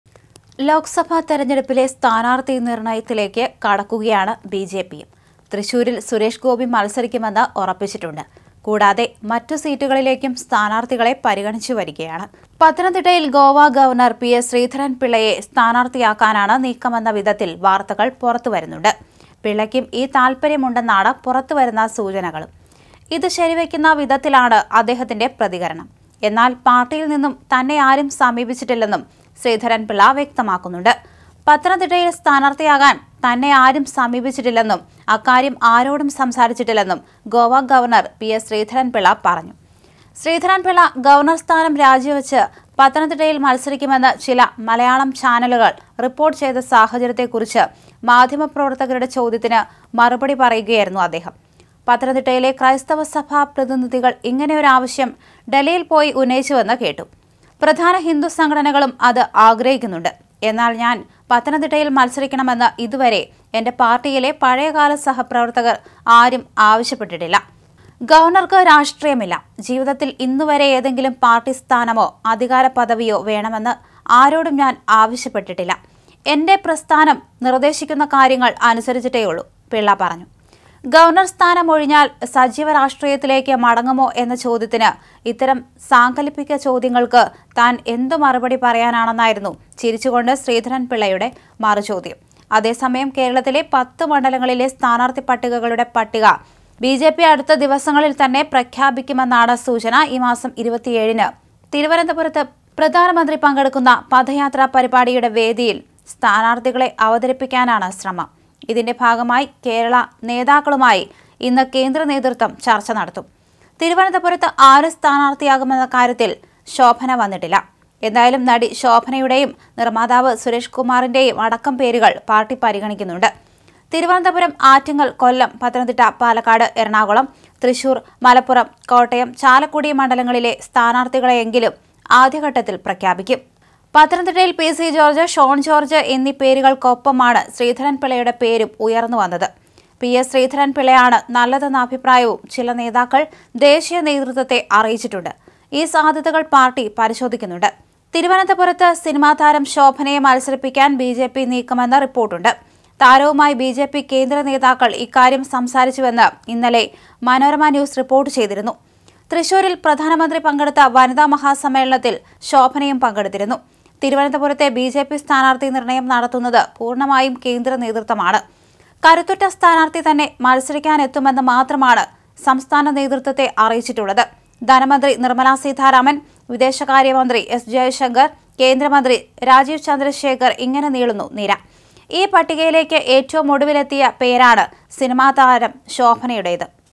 പത്തനംതിട്ടയിലെ ബിജെപി സ്ഥാനാര്‍ത്ഥിയായി പരിഗണിക്കവേ ശ്രീധരന്‍ പിള്ളയുടെ പ്രതികരണം
സഭാപ്രതിനിധികളും ഹിന്ദു സംഘടനകളും ഞാൻ സ്ഥാനാർത്ഥിയാകണമെന്ന് ആഗ്രഹിക്കുന്നുണ്ട്; പാർട്ടിയിൽ നിന്നും തന്നെയാരും സമീപിച്ചിട്ടില്ല; എന്റെ പ്രസ്ഥാനം നിർദേശിക്കുന്ന കാര്യങ്ങൾ അനുസരിച്ചിട്ടേയുള്ളൂ; പത്തനംതിട്ടയിലെ ബിജെപി സ്ഥാനാർത്ഥിയായി പരിഗണിക്കവേ ശ്രീധരൻ പിള്ളയുടെ പ്രതികരണം; പത്തിടത്ത് സ്ഥാനാർത്ഥികളെ പ്രഖ്യാപിക്കാൻ ബിജെപി